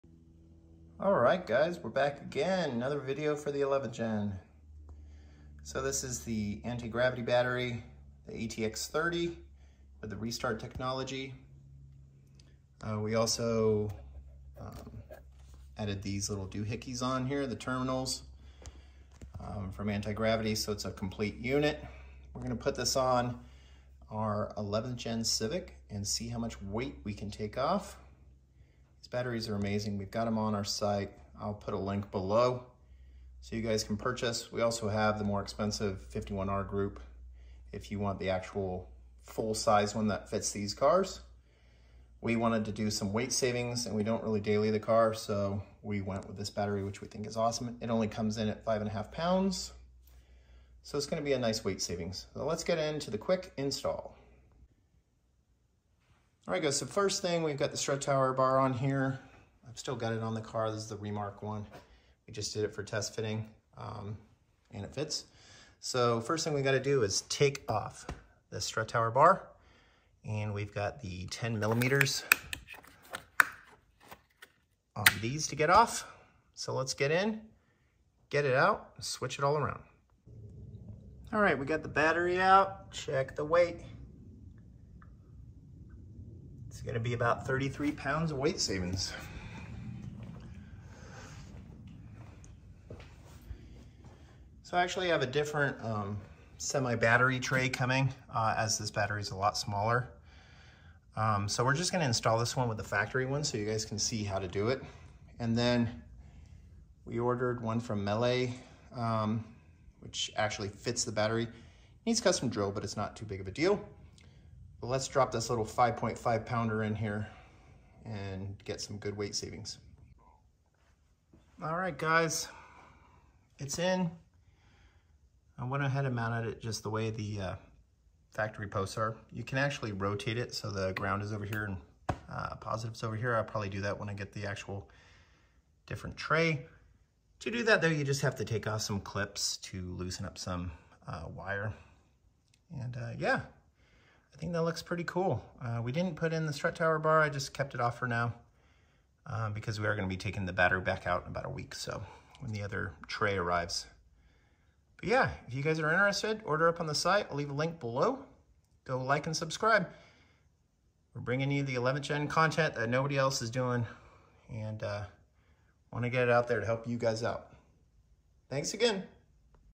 Antigravity Battery install 22 Civic, saved 33lbs of weight
Make sure to get the terminals if you dont have any